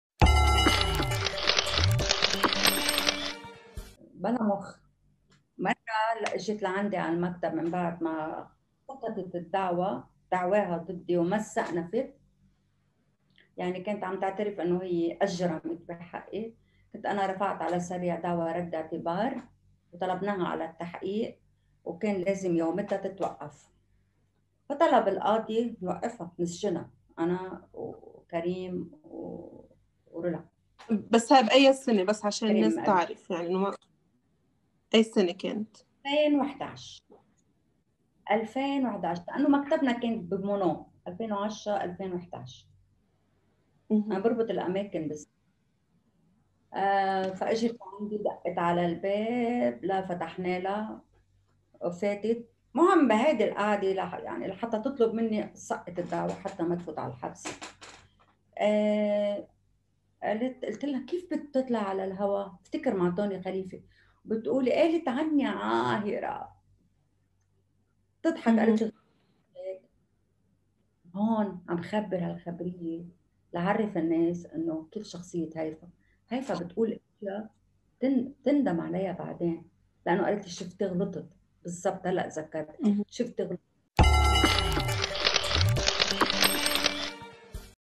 نضال الأحمدية: هيفاء وهبي بلا مخ
قضية شغلت الرأي العام اللبناني والعربي، الحركة المعيبة لهيفاء وهبي عقب تصريح ابنتها زينب فياض بها ما دفع البعض للقول بإنها تقصد ابنتها ولا تريدها ولا تعترف بها

نضال الأحمدية تفشي أسرارًا حول قضية هيفاء وابنتها زينب، وتقول ما لا يعرفه أحد لأنها نشرته كله  في مطلع التسعينات. هنا مقطع صغير من الحوار الكامل الذي ينشر يوم السبت.